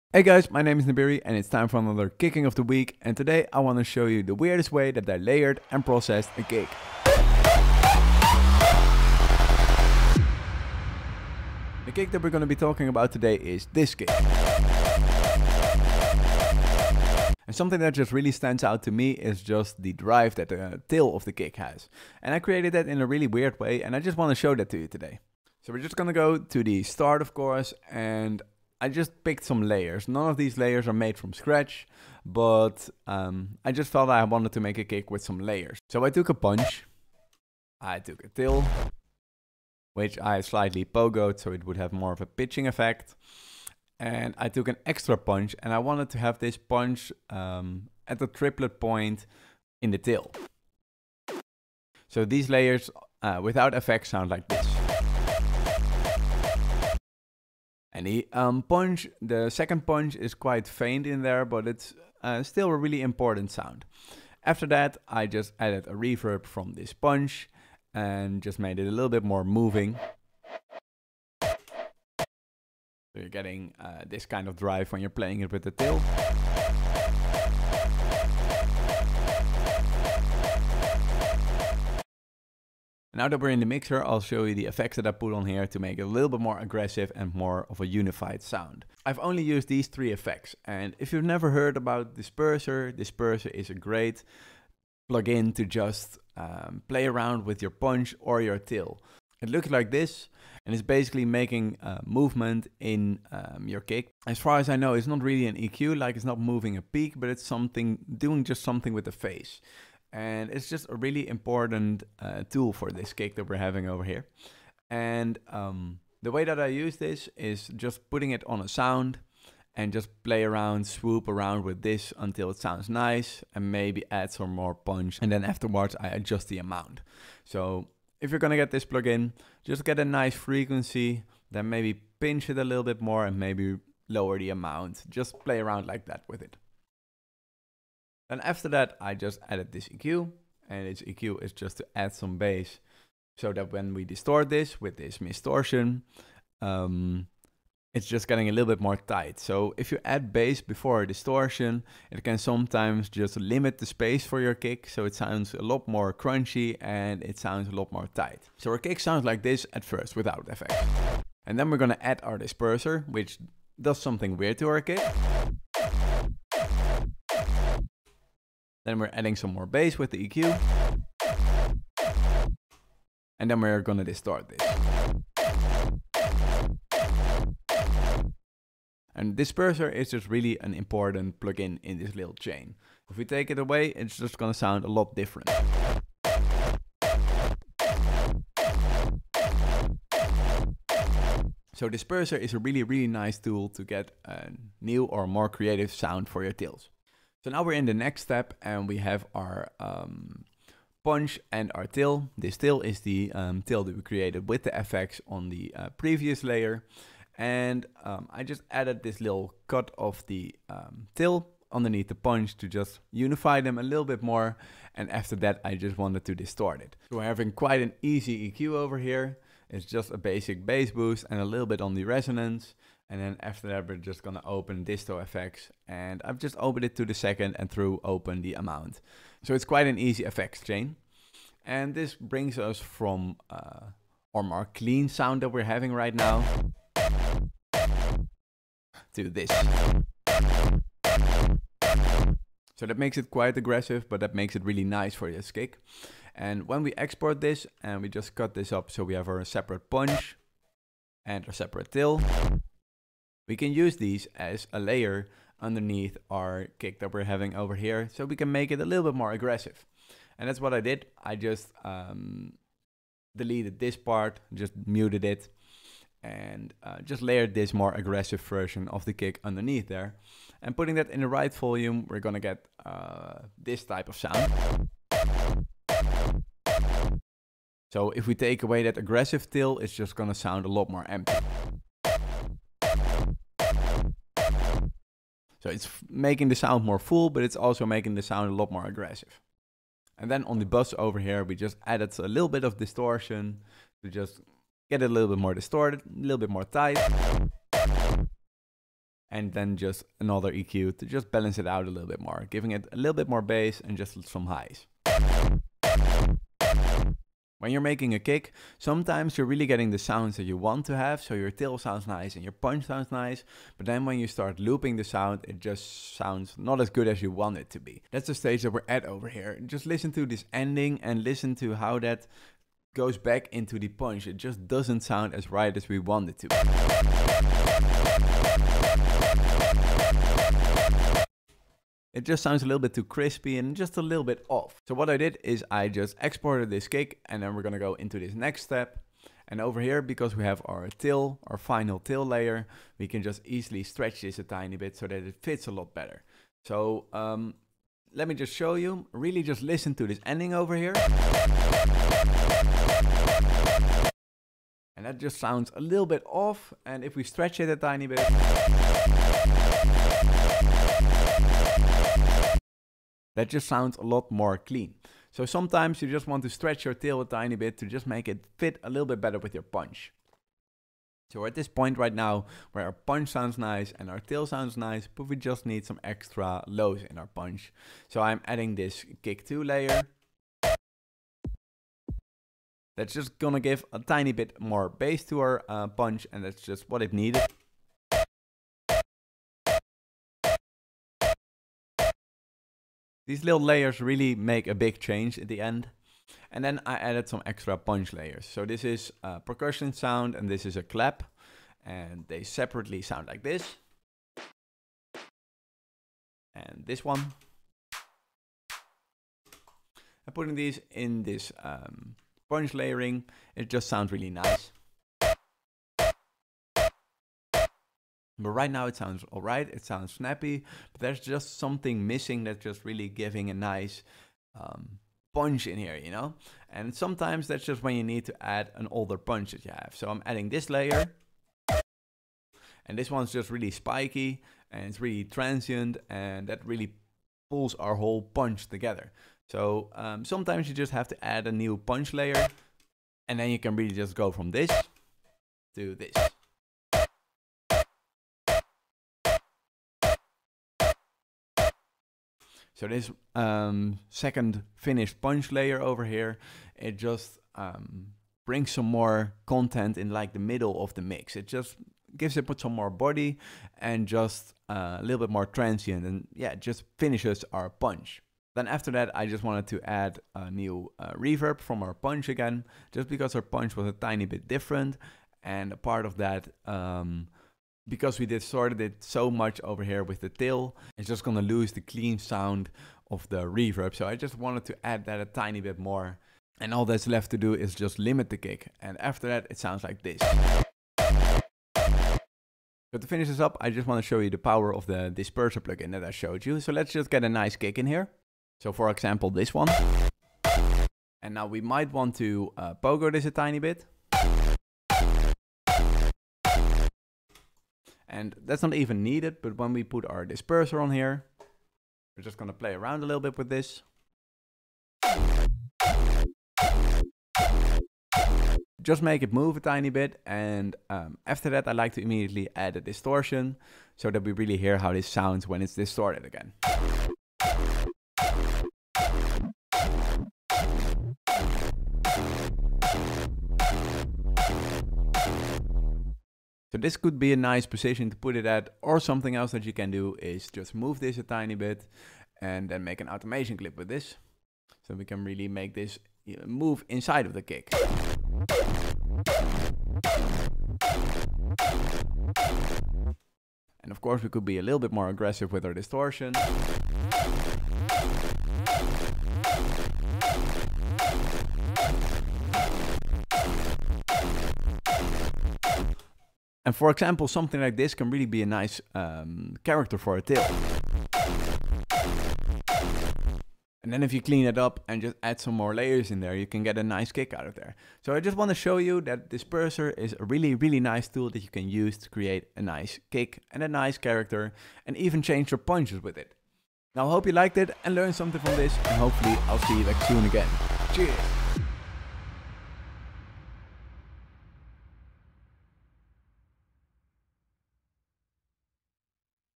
This is the STRANGEST WAY I've made a kick...
In this video I want to show you the power of one of my favourite plugins: Disperser! It's just such a nice tool that can shape and re-shape a kick entirely!

Timestamps:
00:00 Introduction
00:25 Creating the character and drive of the kick
03:16 Making the tail sound more full
05:07 Fixing the drive and finishing the kick
08:13 How I like to work with Disperser